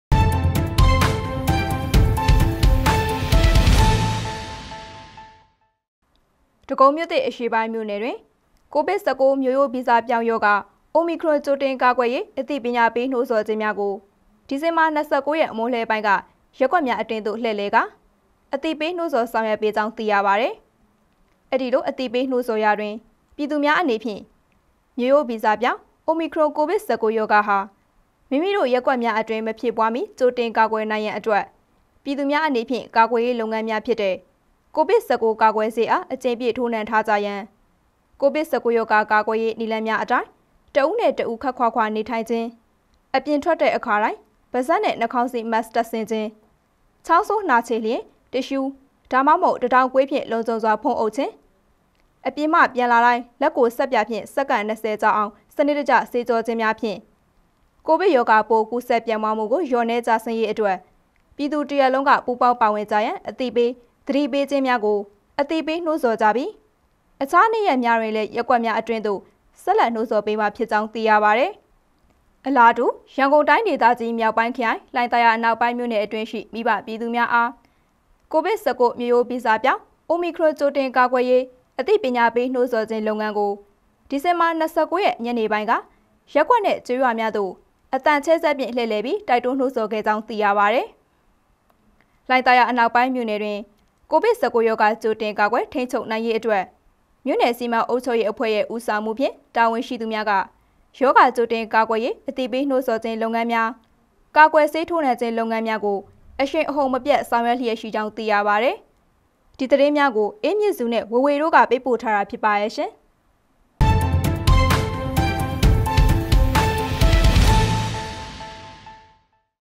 အိုမီခရွန် ကြိုတင်ကာကွယ်ရေးအသိပညာပေး နှိုးဆော်ဆောင်ရွက်ခဲ့ကြောင်းသိရ
Shwe Yangon Media

ကိုဗစ်-၁၉ မျိုးရိုးဗီဇပြောင်းရောဂါ
အိုမီခရွန် ကြိုတင်ကာကွယ်ရေး
အသိပညာပေး နှိုးဆော်ဆောင်ရွက်
ခဲ့ကြောင်းသိရ